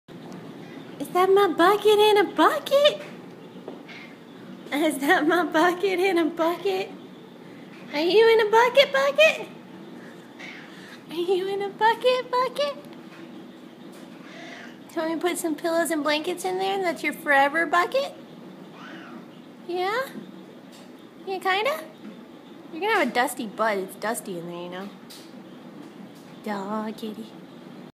Bukut in a Bucket
My cat named Bukut jumped in this plastic bucket of his own free will and so I had to take advantage of the moment!